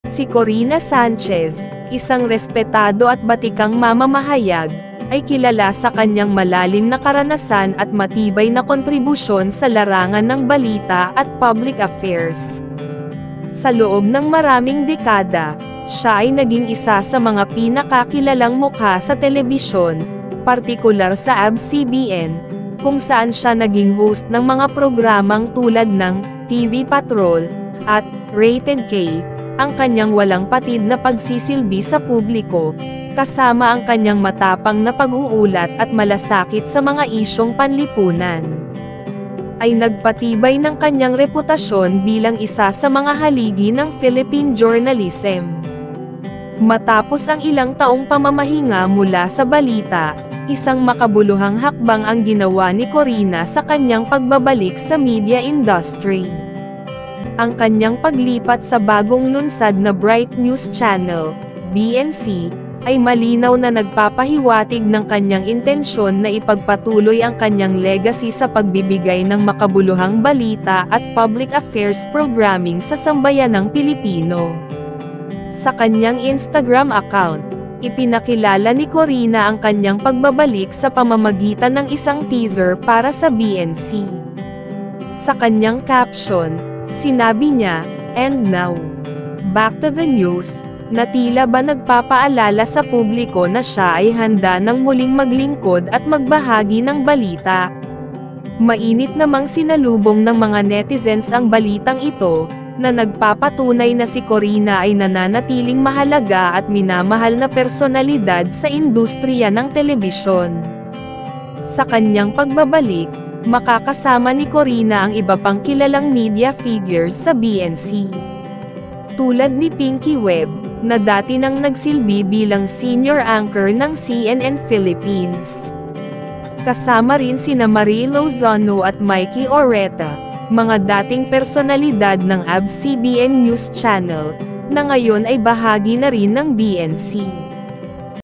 Korina Sanchez, Muling babalik sa Pagbabalita!
Korina Sanchez, ang batikang mamamahayag, ay nagbabalik sa telebisyon upang muli tayong ihatid sa mga makabuluhang balita at kwento sa bagong yugto ng kanyang karera.